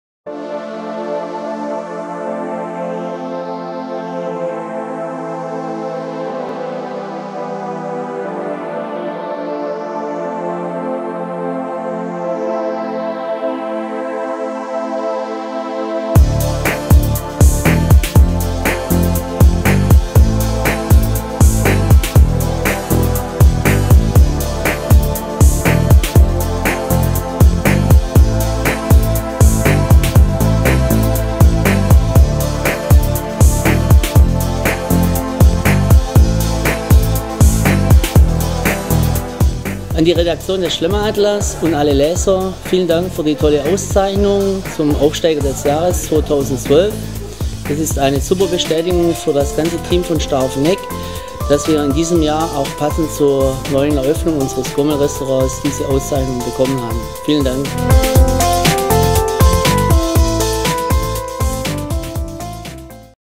Rolf Straubinger - Aufsteiger des Jahres 2013
Seit Rolf Straubinger im Jahr 1979 seine Ausbildung zum Koch begann, führte ihn sein beruflicher Weg kontinuierlich nach oben. Sein kreatives Zentrum hat er seit einigen Jahren im Burgrestaurant Staufeneck gefunden. Hier kredenzt er Symbiosen von regionalen und saisonalen Produkten mit Gewürzen aus aller Welt. Besonders für seine ideenreichen Fischgerichte ist er bei Gourmets bekannt und beliebt. Für seine einfallsreichen Kreationen erhielt er vom Schlemmer Atlas auf der 15. BUSCHE GALA  den Titel „Aufsteiger des Jahres 2013".